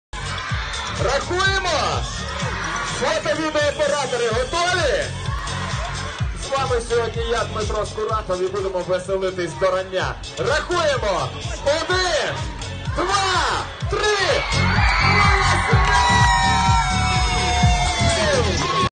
У Ясінях пройшла масштабна ековечірка (1)
Екологічне свято в Ясінях відбулось уже вдруге.
Головною метою заходу, за словами організаторів, не лише привернути увагу односельчан до проблем екології та сортування сміття зокрема, але і показати, що процес збереження природних ресурсів мрже бути веселим та захоплюючим.